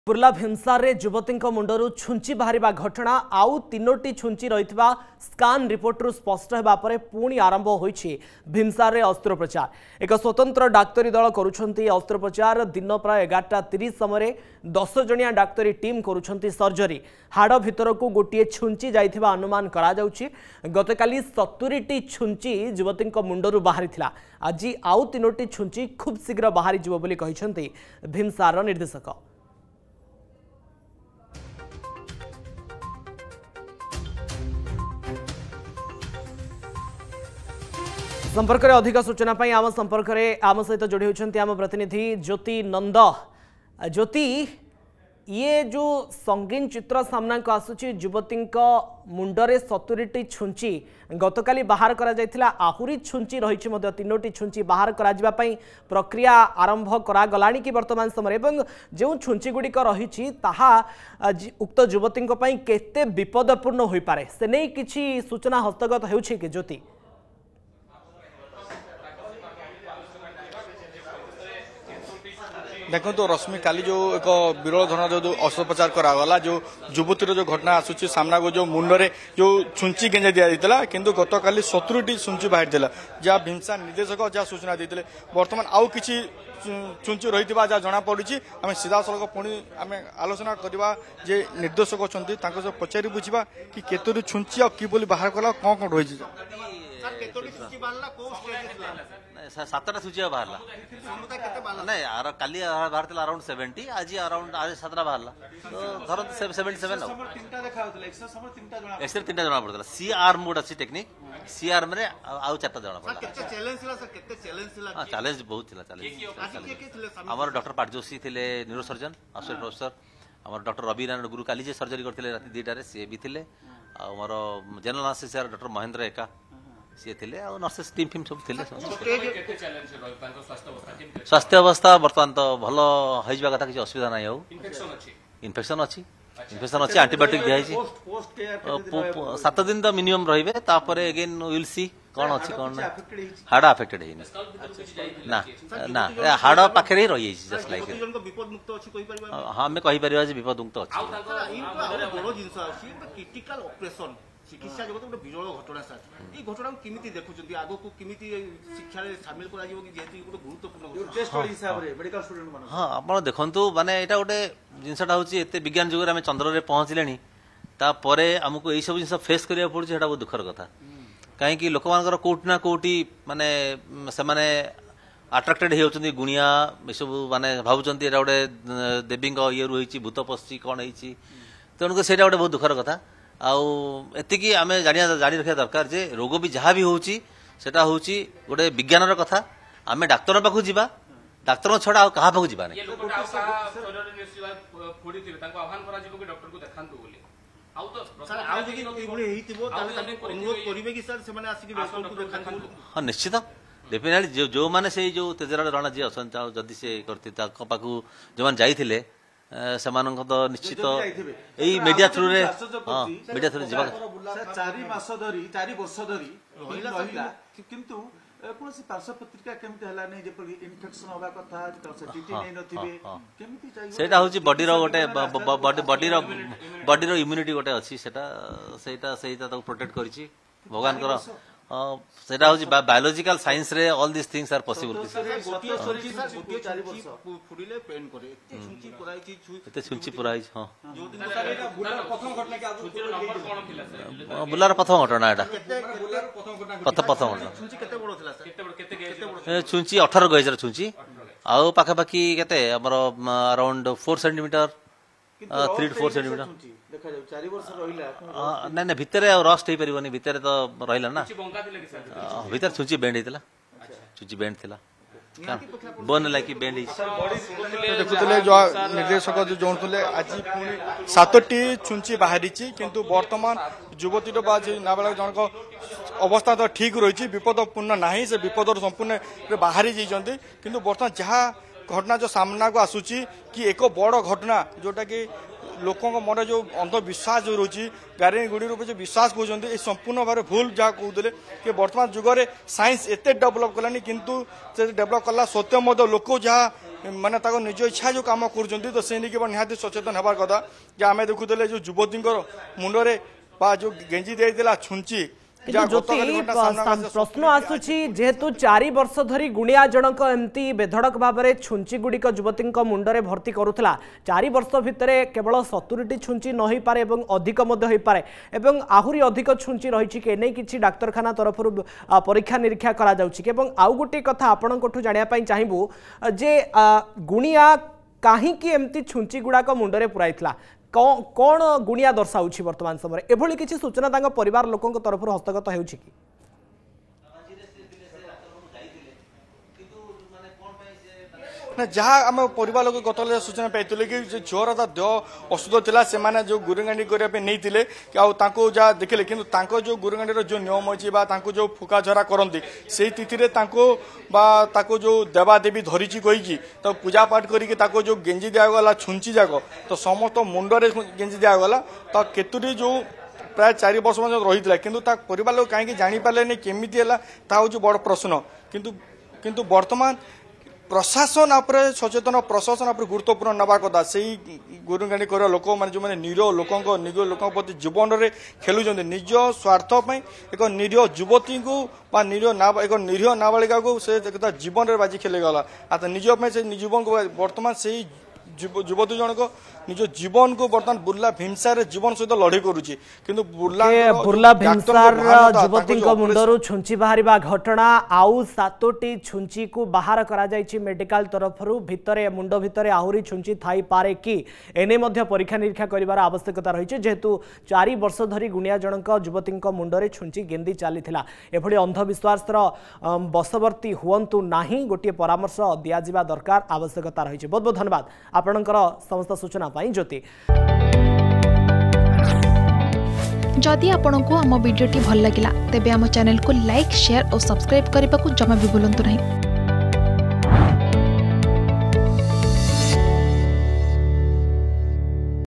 ବୁର୍ଲା ଭିମସାରରେ ଯୁବତୀଙ୍କ ମୁଣ୍ଡରେ ଆହୁରି ରହିଛି 7ଟି ଛୁଞ୍ଜି || Burla Bhimsar
ବୁର୍ଲା ଭୀମସାରରେ ଯୁବତୀଙ୍କ ମୁଣ୍ଡରୁ ଛୁଞ୍ଚି ବାହାରିବା ଘଟଣା । ଆଉ ୭ଟି ଛୁଞ୍ଚି ରହିଥିବା ସ୍କାନ୍ ରିପୋର୍ଟ ରୁ ସ୍ପଷ୍ଟ ହେବା ପରେ ପୁଣି ଆରମ୍ଭ ହୋଇଛି ଭୀମସାରରେ ଅସ୍ତ୍ରୋପଚାର l